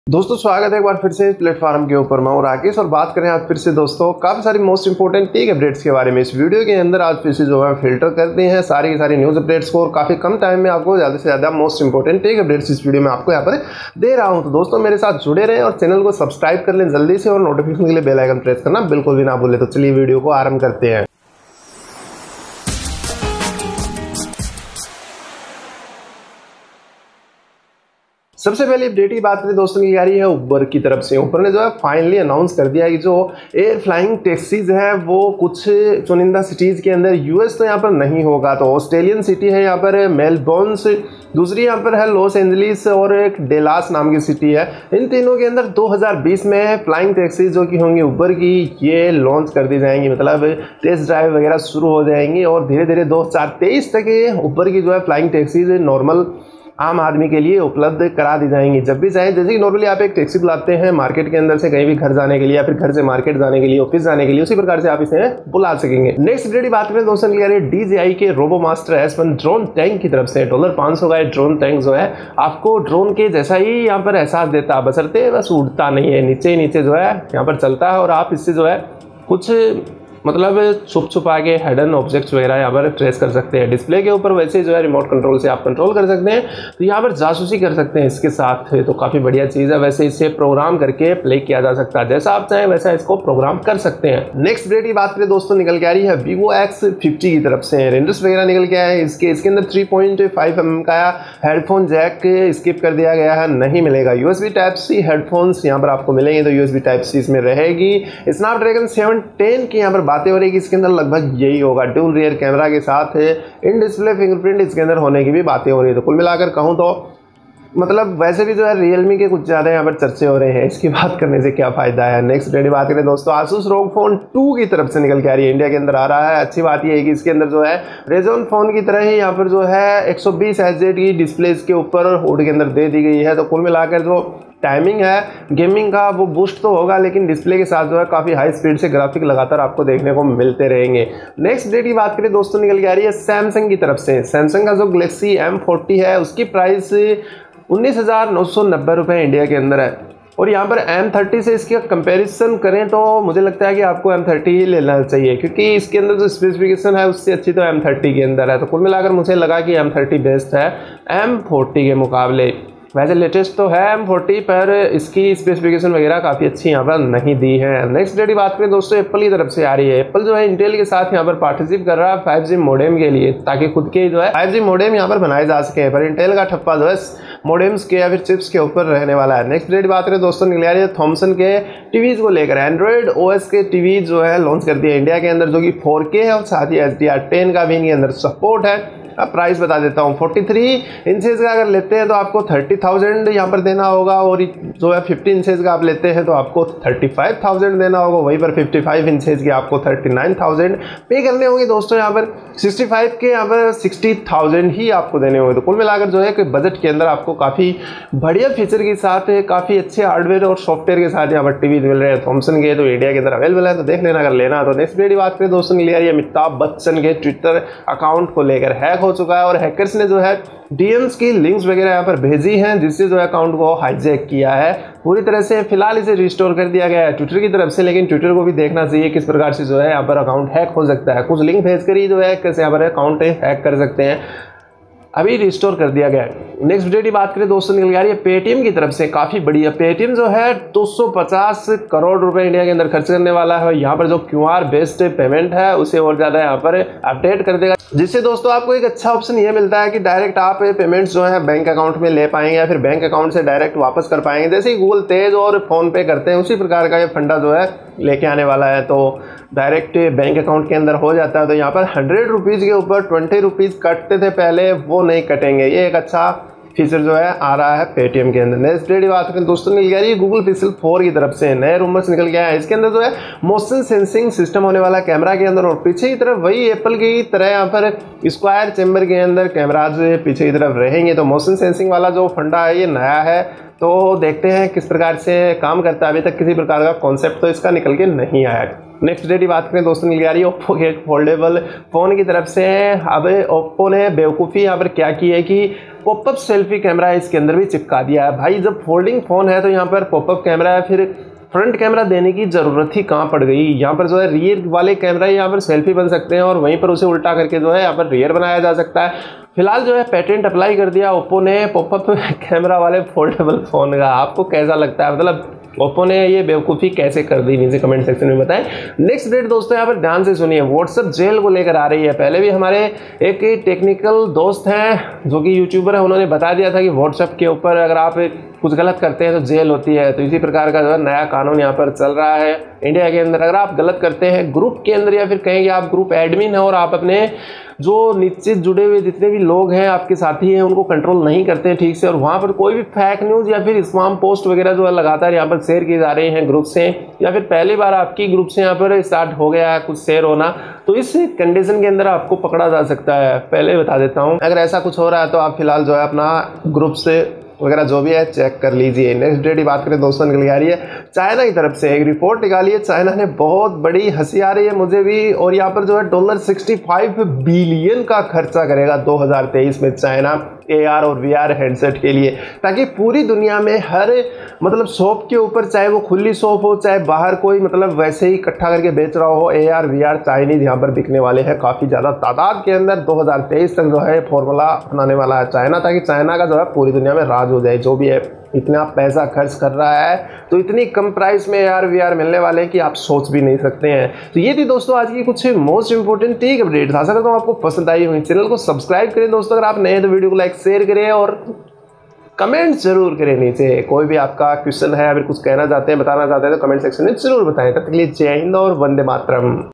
Tech Up's #460 - Whatsapp Jail, DJI drone tank, Uber Flying Taxis, ROG phone 2 , Pixel 4
Namskar Dosto Is video Mein Maine aaj fir se baat ki hai bahut sari awesome tech news ke bare mein jaise: Uber Air Finally Trial Flying Taxis, DJI RoboMaster S1 drone tank, Asus ROG phone 2, Thomson new android TVs in India, Amitabh Bachchan Twitter Hack, Paytm will invest Rs 250 crore, Oppo foldable phone with pop-up camera, Whatsapp Jail – Group Spam Post, China spend over $65 billion and more!

Technique Gyan Welcomes You & Your Family!

My Camera
My Mic
My Tripod
My Lights
Video Edit - adobe premiere pro cc
My Laptop
My headphone